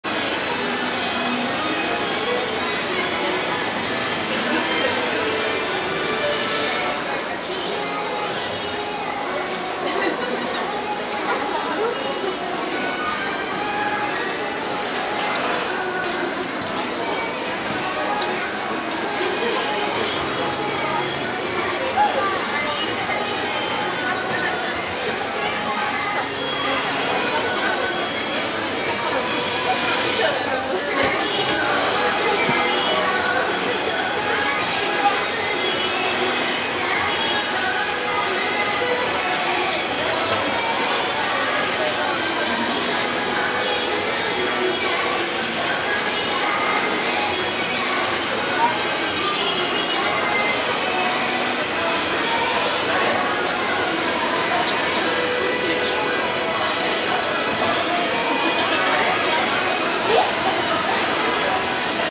ランディングしようか悩むララさんとランちゃん❓ 鴨川シーワールド 2024年2月23日 00007
鴨川シーワールドにて
2024年2月23日